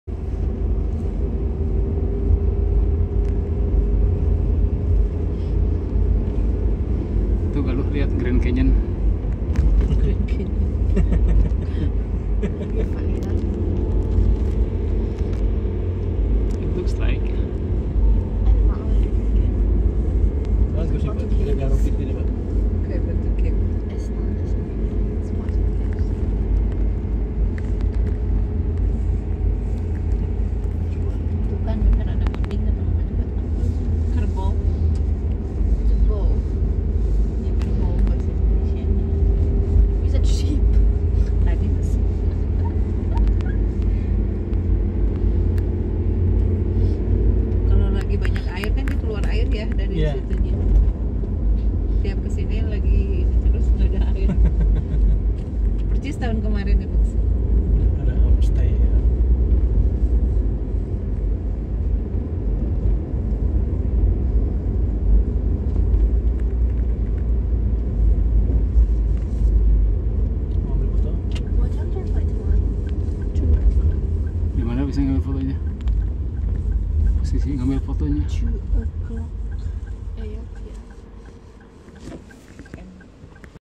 Driving Around Hara - West Sumatera
Perjalanan di daerah Wisata Hara di Sumatera Barat